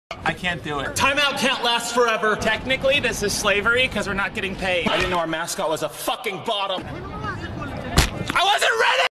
The children in Ninjago DR in a Nutshell *SPOILERS FOR S2P2* •Ninjago Skit•
I don't own Characters or Ninjago: owned by LEGO

Characters:
Pink Furry Child: Sora
Feisty Feral Child: Wyldfire
Traumatized Green Child: Lloyd
Child going through villain/emo phase: Arin
Red head child that can't get a break so far: Jordana
Glowing yellow ball that can't stop causing problems: Wu